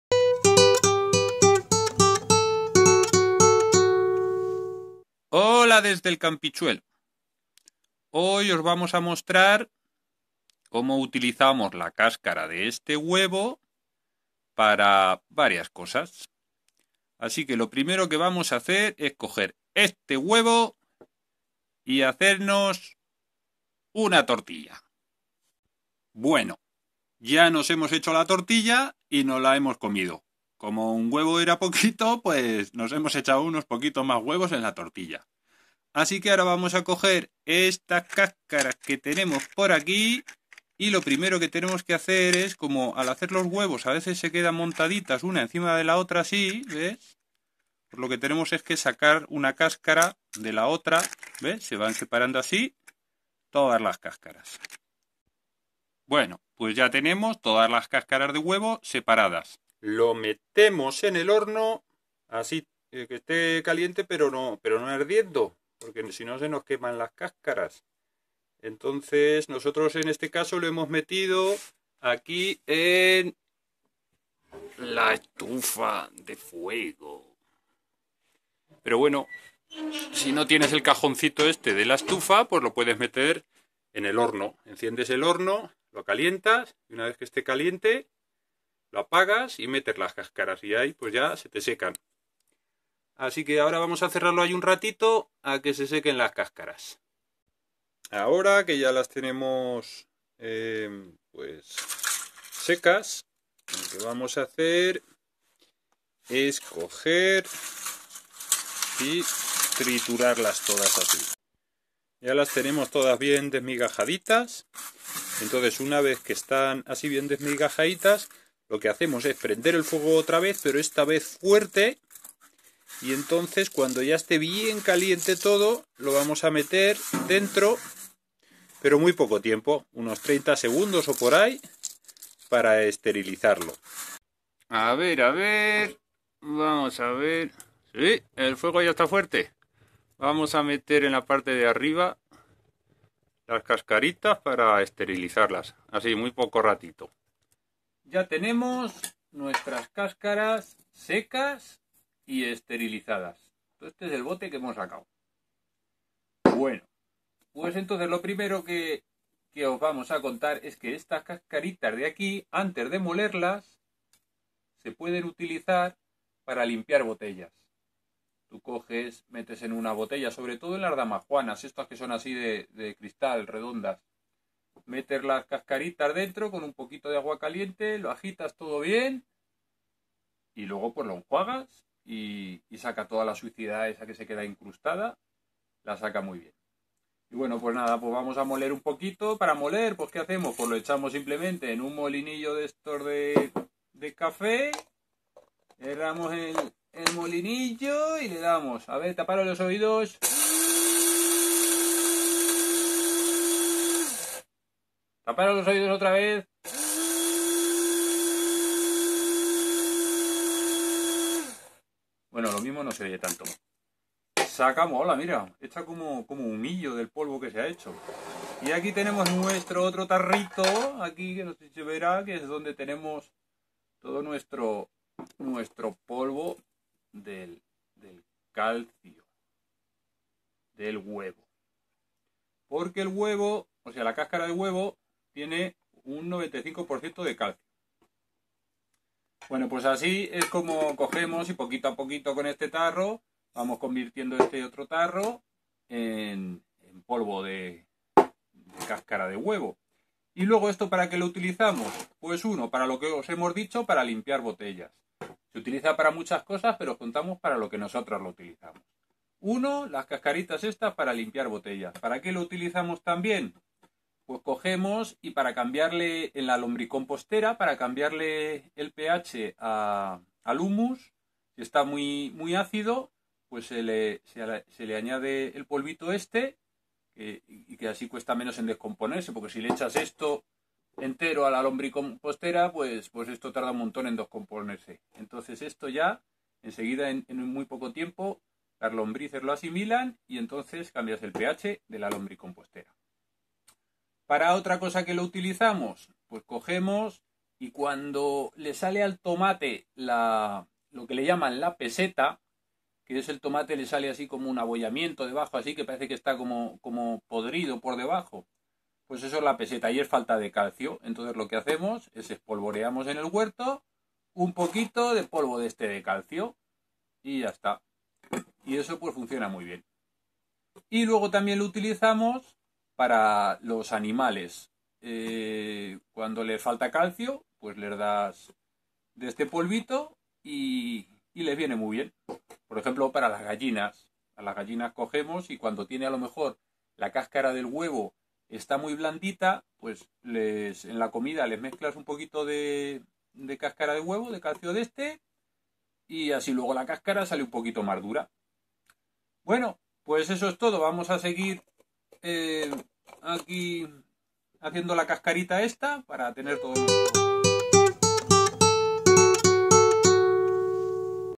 para que sirven las cascaras de huevo
Vídeo de como preparamos las cascaras de huevo para darles varios usos.